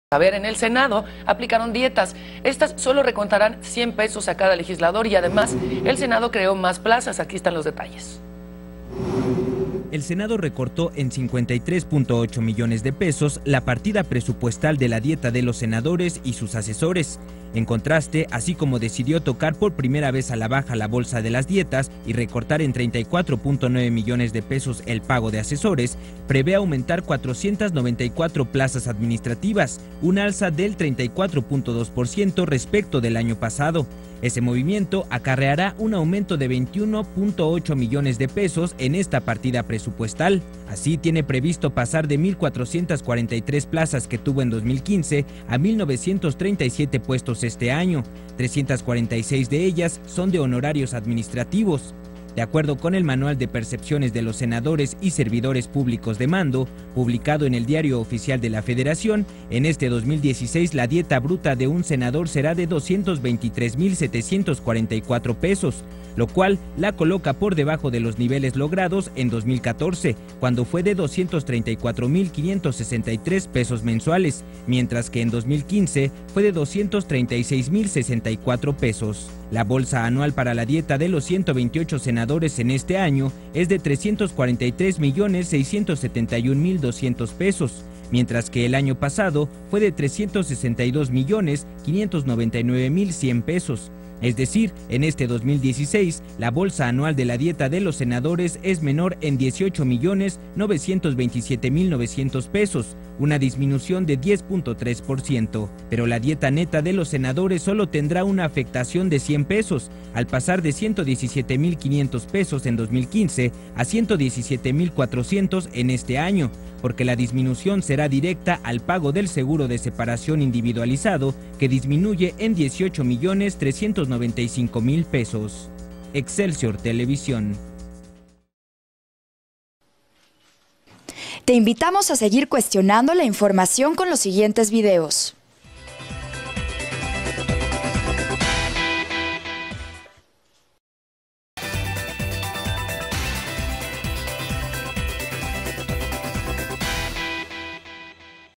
Aplican "dieta" de cien pesos a senadores / Mariana H
El Senado recortó en 53.8 millones de pesos la partida presupuestal de la dieta de los senadores y sus asesores, cada legislador sólo tendrá una afectación de 100 pesos, al pasar de 117 mil 500 pesos en 2015 a 117 mil 400 este año

29 de febrero 2016